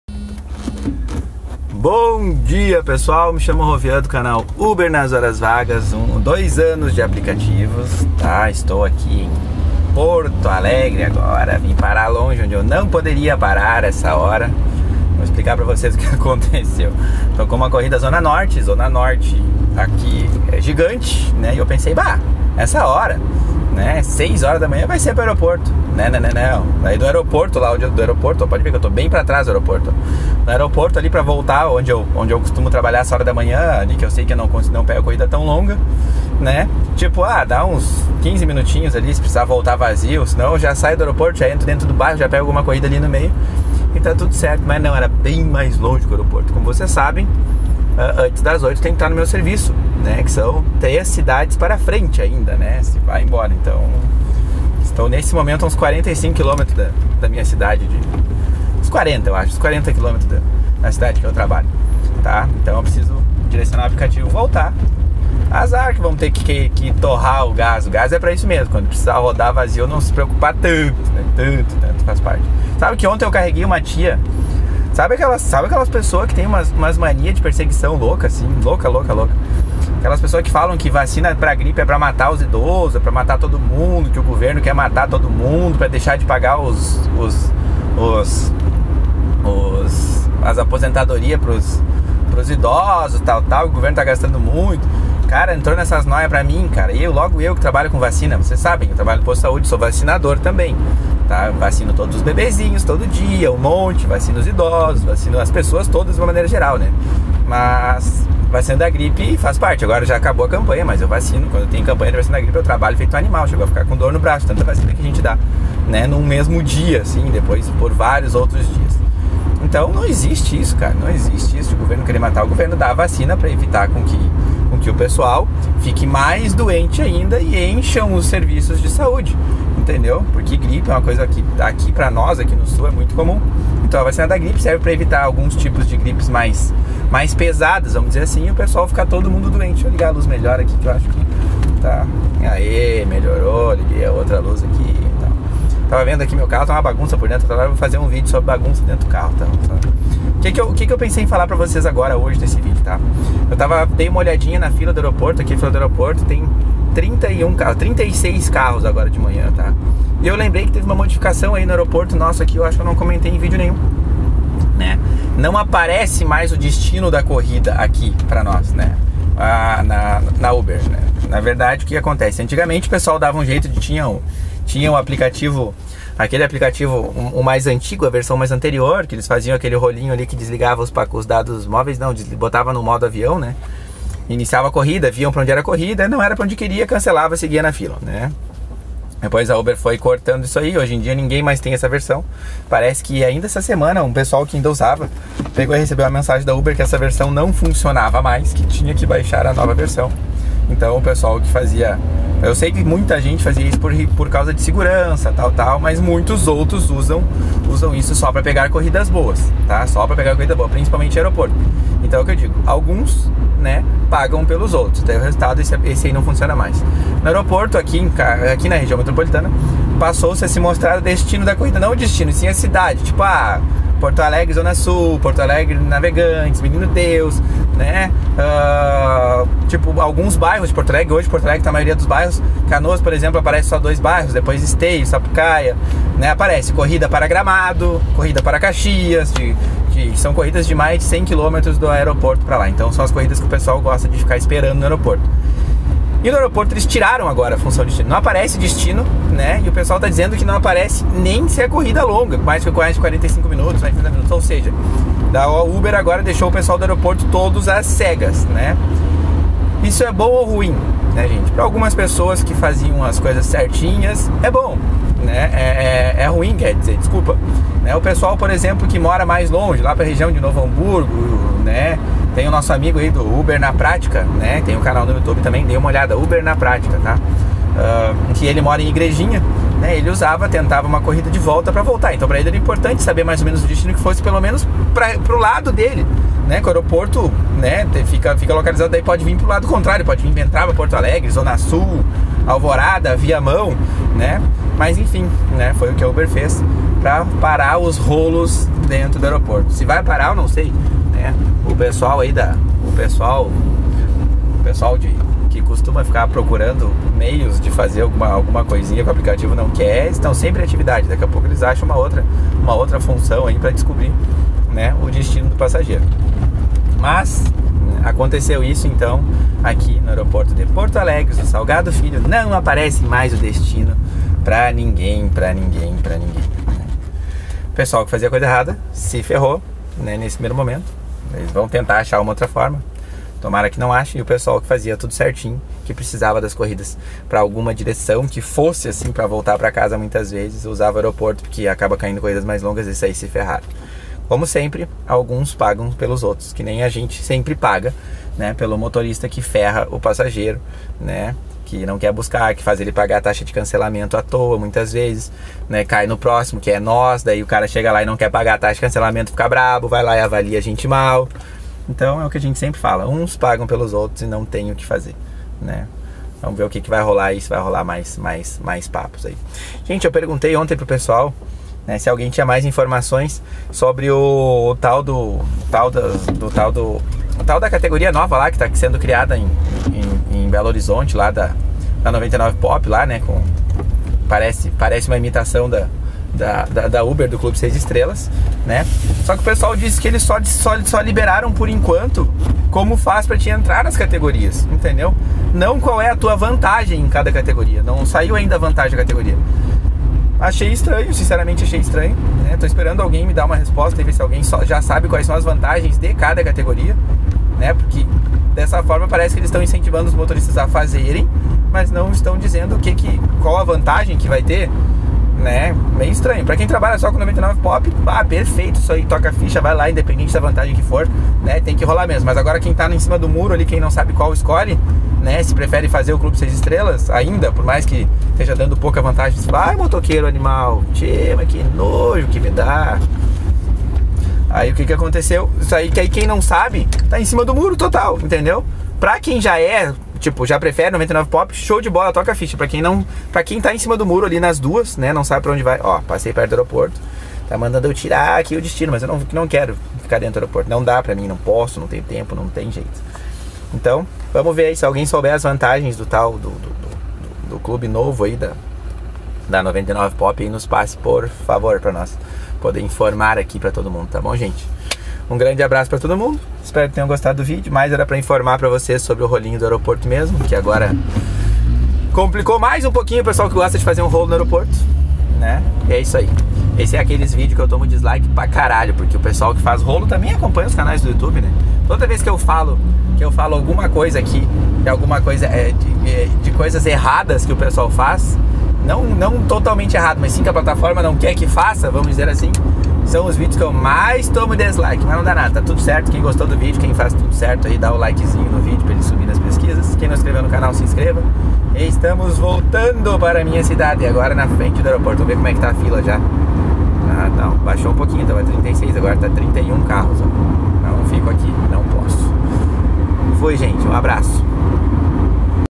Aeroporto! Sempre tem algo mudando! Ninguém mais sabe para onde vai!😲
Aeroporto não mais aparecendo destino como em toda região metropolitana!  E como sempre os bons motoristas pagam pelos ruins.